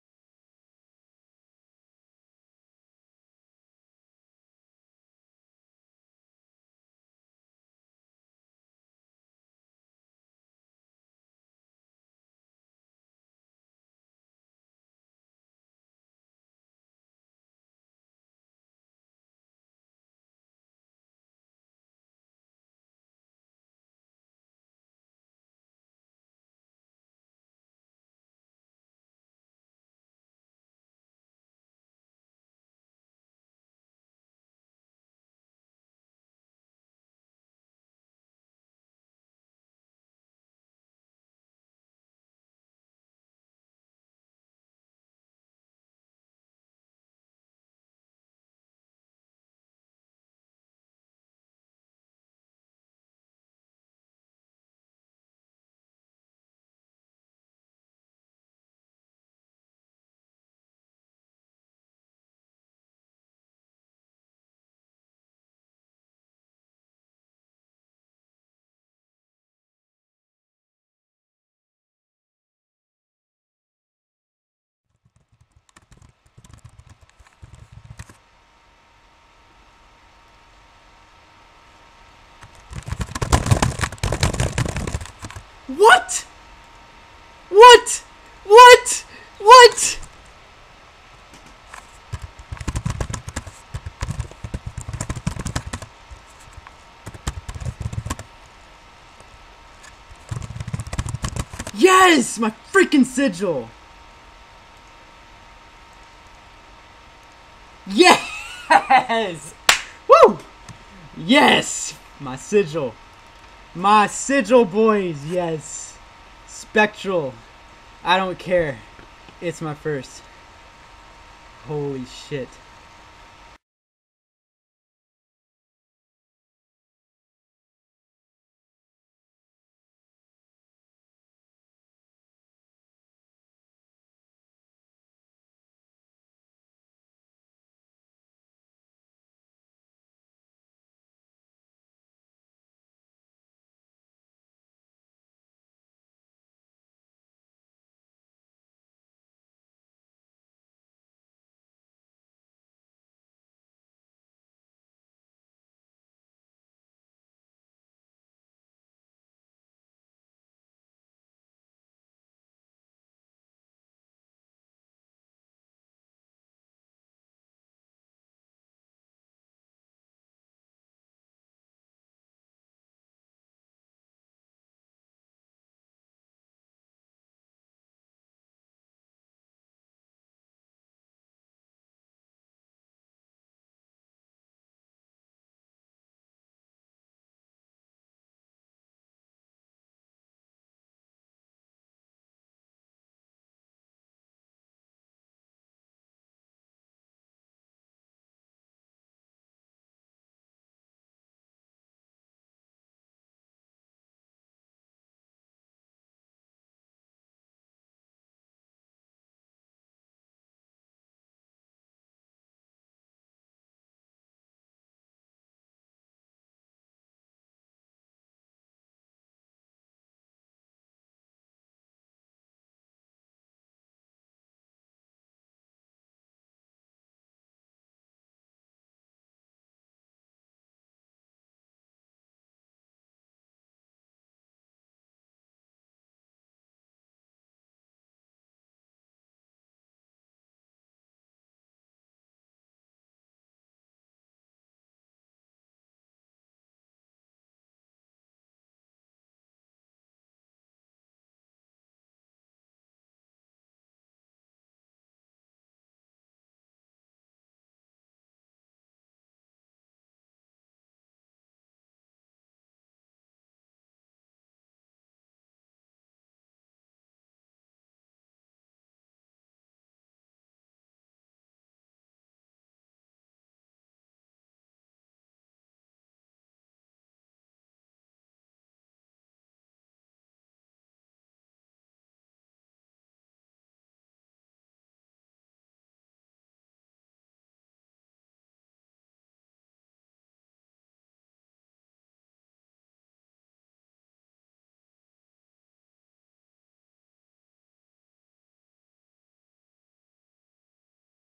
Spectral Sigil Drop My Sigil!!
Finally got my Sigil drop on kc 731
(lower your volume lol Since it's my first sigil drop I had a bit of a nerdgasm)